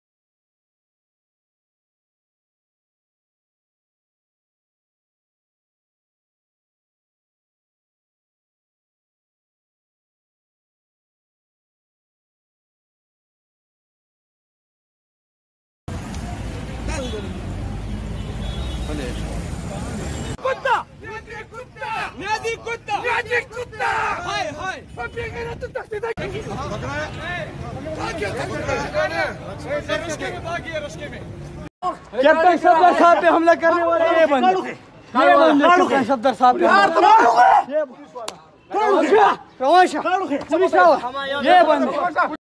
Eggs thrown on PMLN Leader Safdar Awan in Peshawar
کیپٹن صفدر پر پشاور میں انڈوں سے حملہ۔